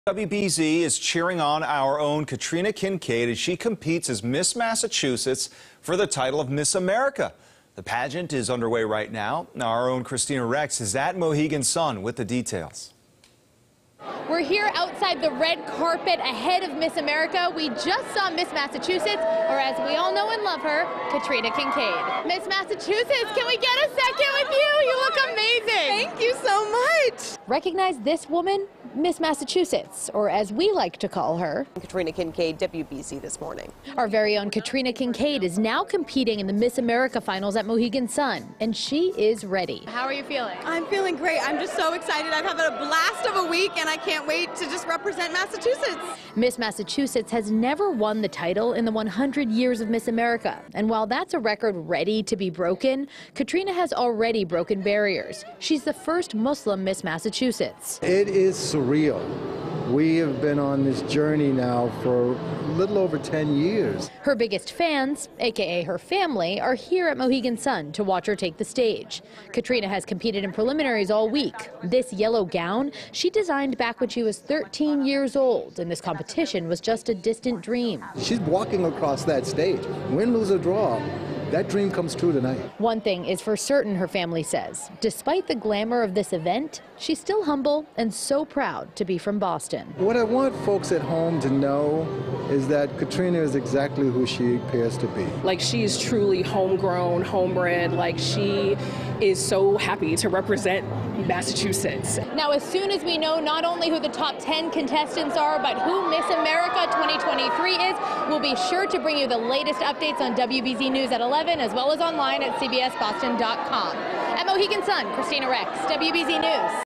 Katrina Kincade competing in Miss America competition
WBZ-TV's own Katrina Kincade is representing the Bay State. WBZ-TV's Kristina Rex reports.